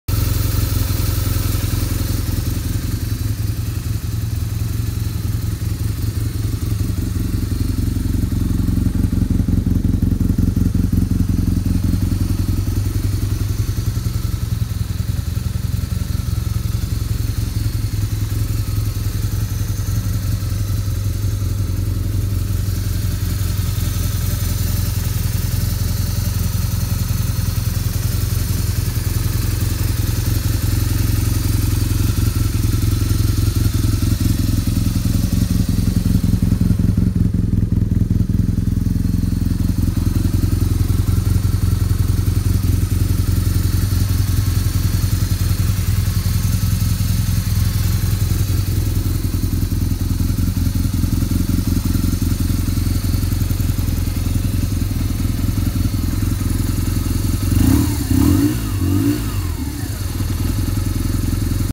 BMW R1200S 2006:56
Lovely condition throughout. 18K on the clock. Ohlins shocks, Remus cans. FSH. £6200. Finance and Delivery available.